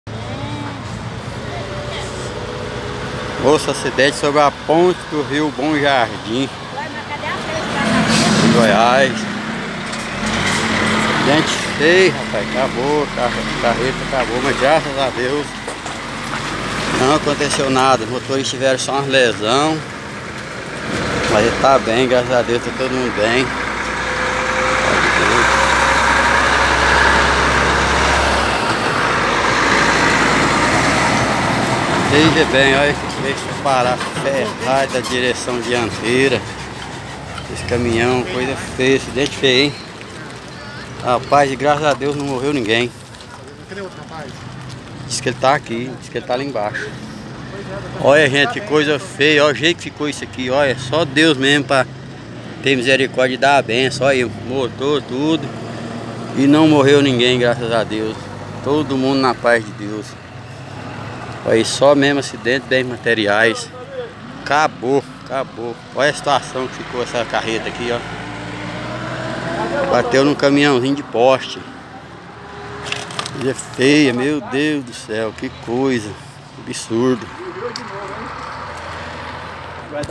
Acidente entre caminhões na ponte sobre o rio Bom Jardim
Aconteceu nesse sábado (10) um acidente na ponte sobre o rio Bom Jardim, próximo à cidade de Bom Jardim de Goiás entre uma carreta e um caminhão que carregava postes. A carreta acabou tombando, porém nenhum dos envolvidos se feriu gravemente, sendo apenas o motorista com algumas leves lesões.